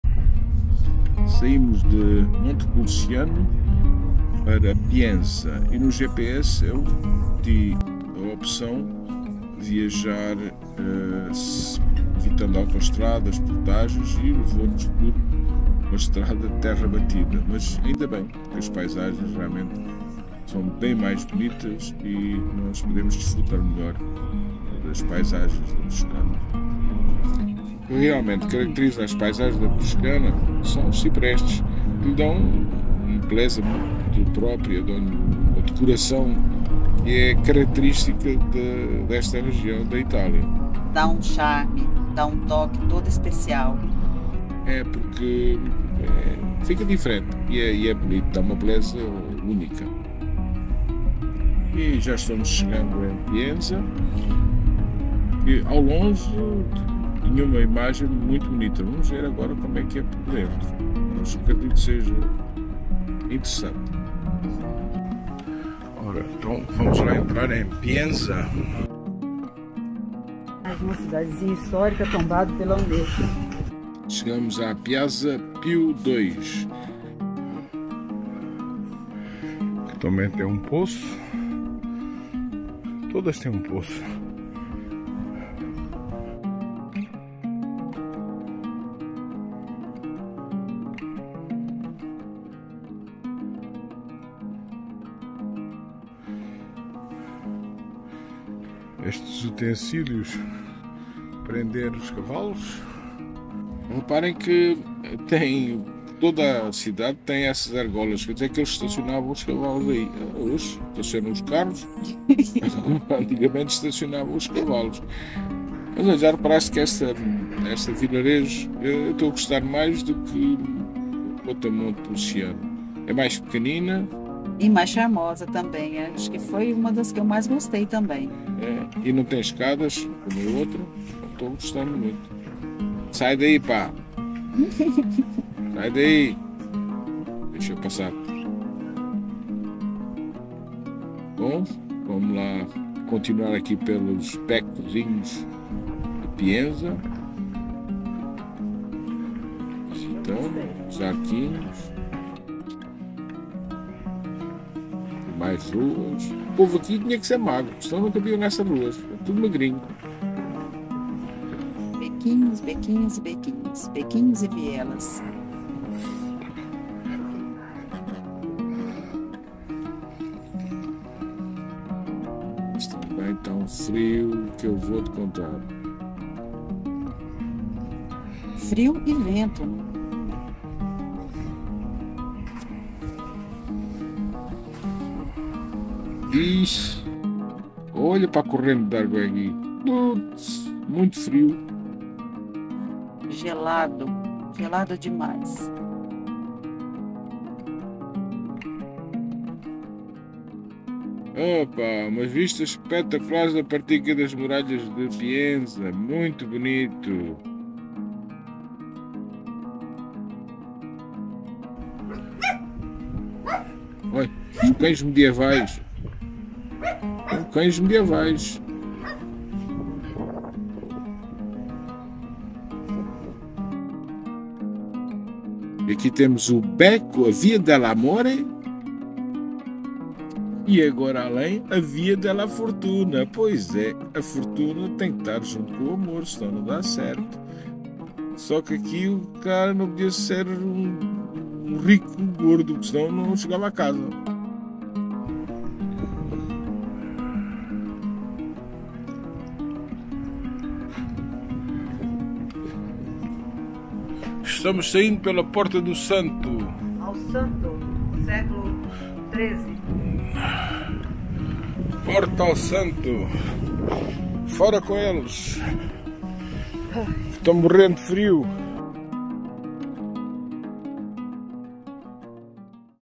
26º dia - PIENZA, Toscana - Viajando pela Europa em autocaravana (MotorHome)
No nosso 26º dia também visitamos Pienza e deixamos aqui o vídeo da cidade.
Foi uma das cidades históricas que gostamos bastante apesar de não ser muito grande. Mais vídeos da Toscana no nosso canal, subscreva para não perder nenhum